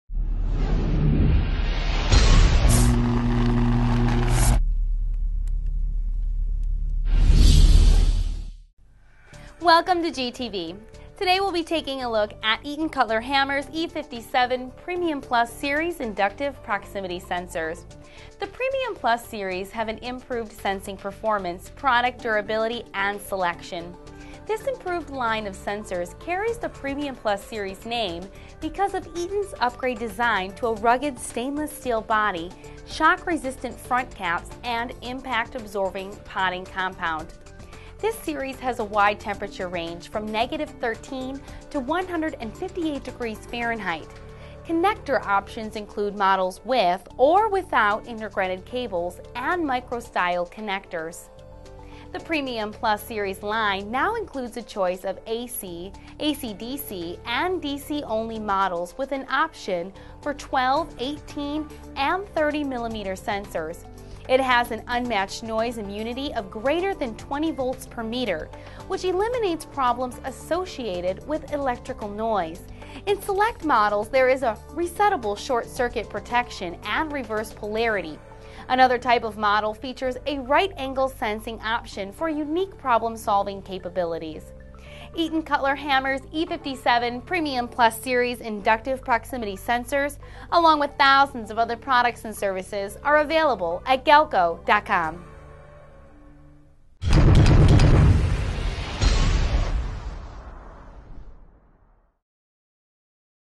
Eaton Cutler-Hammer Inductive Proximity Sensor Overview - E57 Premium Plus Series
Eaton Cutler-Hammer E57 Premium Plus Series Inductive Proximity Sensor on Galco TV.

Eaton's Premium Plus Series Proximity Sensors have improved sensing performance, product durability and selection. These sensors are ideal for automation, robotics, transfer lines, conveyors, and material handling.

-    Shielded or Unshielded
-    NPN or PNP Output Available
-    Cable, Micro, Micro Pigtail, or Mini Connectors
-    LED Status Indicator
-    CE Approved (AC/DC and DC-Only Models)

E57 PREMIUM + Series Features:
-    2~22mm Sensing Range
-    2 or 3 Wire
-    20~250VAC or 6~48VDC
-    12~30mm Diameter
-    Right Angle or End Sensing

And to view the full line of Eaton Cutler-Hammer products available at galco.com, please follow this